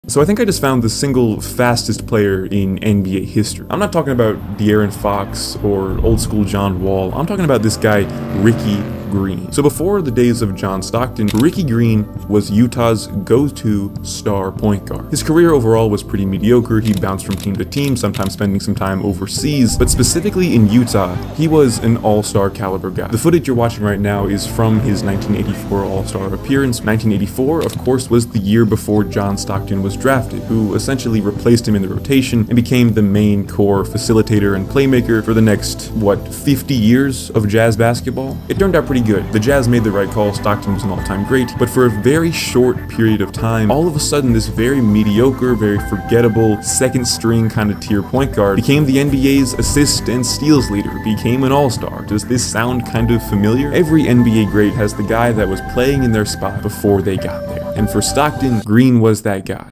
Meet the FASTEST NBA Player EVER.
Utah Jazz point guard Rickey Green, the all-star guard that came before John Stockton, might just be the fastest player in NBA history.